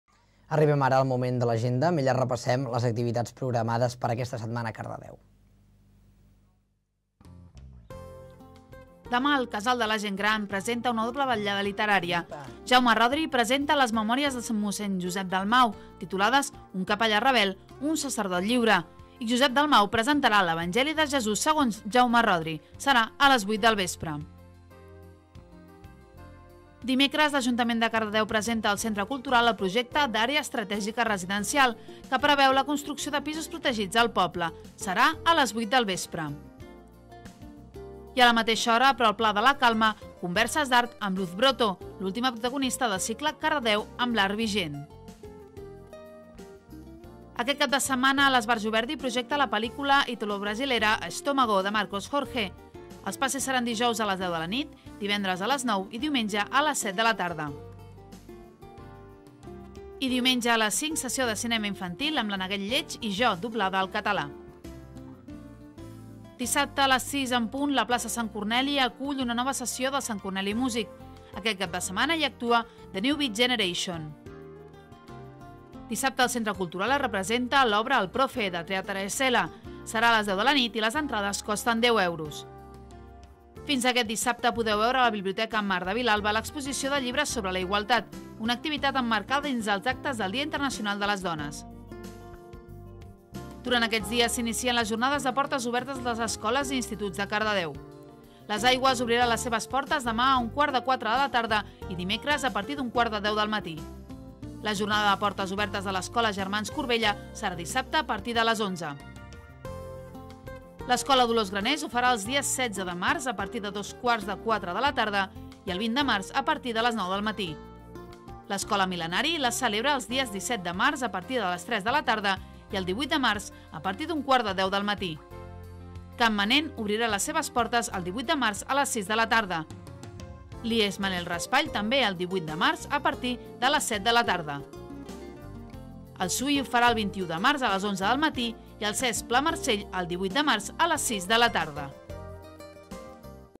09 Les propostes de la setmana
Arribem ara al moment dedicat a lagenda, amb ella repasssem ara les activitats més destacades daquesta setmana.